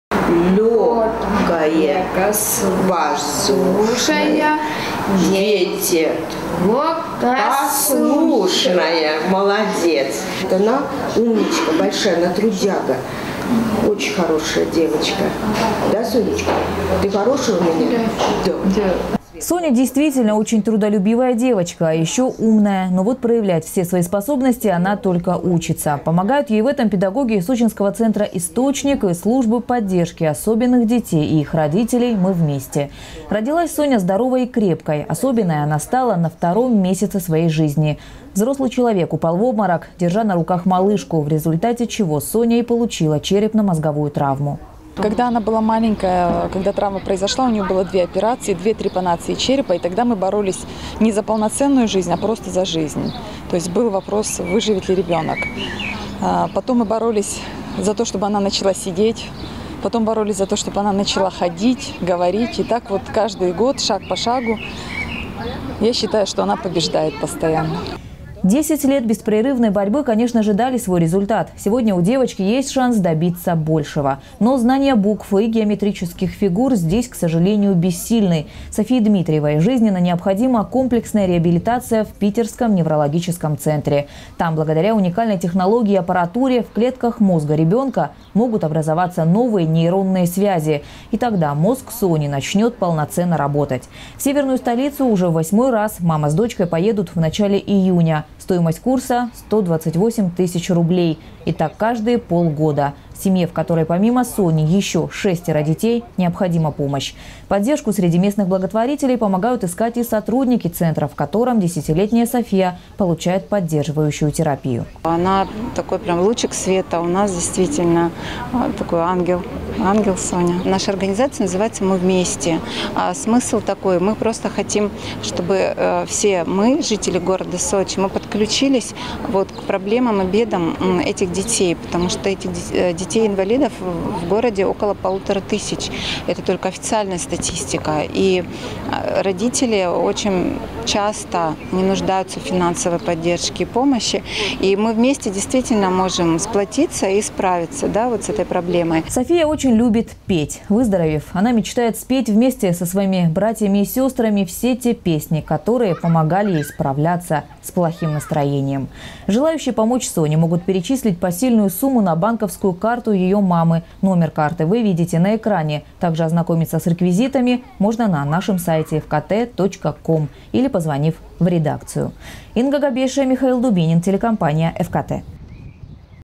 Десятилетней сочинке Соне Дмитриевой нужна срочная помощь Новости Сочи Эфкате
В двухмесячном возрасте девочка получила тяжелую травму головы. Пережив различные операций и две трепанации черепа, Соня научилась ходить и даже немного говорить. Чтобы не остановиться на достигнутом, девочке необходима дорогостоящая реабилитация.

Соня, действительно, очень трудолюбивая девочка. А ещё умная. Но вот проявлять все свои способности она только учится. Помогают ей в этом педагоги сочинского центра «Источник» и службы поддержки особенных детей и их родителей «Мы вместе». Родилась Соня здоровый и крепкой. Особенной она стала на втором месяце своей жизни. Взрослый человек упал в обморок, держа на руках малышку, в результате чего Соня и получила черепно-мозговую травму.

Сочи. Новости.
Телеканал Эфкате.